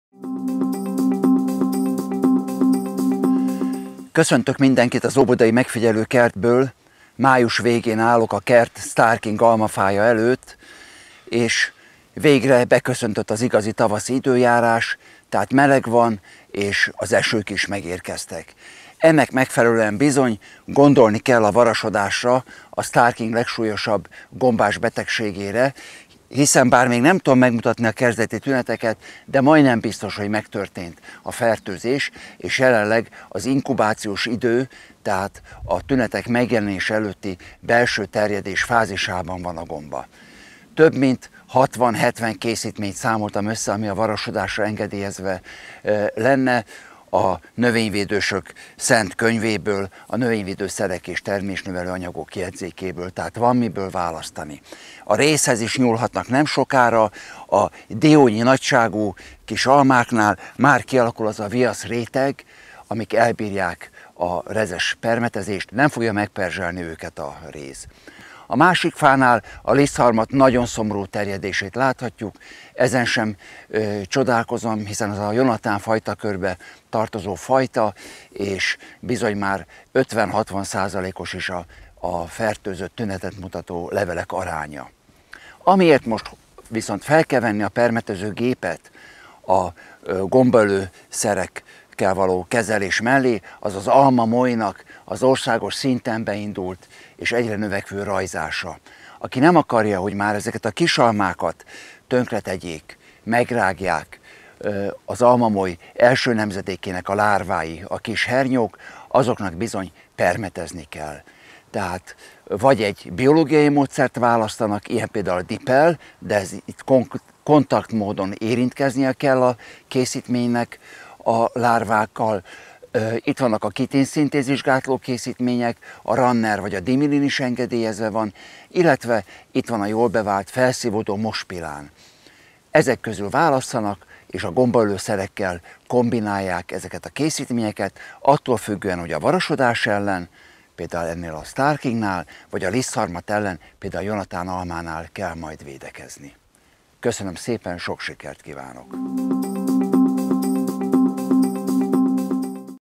Alma: Várhatóan erős lisztharmat fertőzéssel kell szembenézzünk
Május végén a varasodásra érzékeny fajtáknál meg kell kezdeni a védekezést.Több mint 40 szert lehet bevetni. Jonatán fajtákon tombol a lisztharmat. De a gombaölős védekezések mellé be kell tenni almamoly elleni készítményeket is.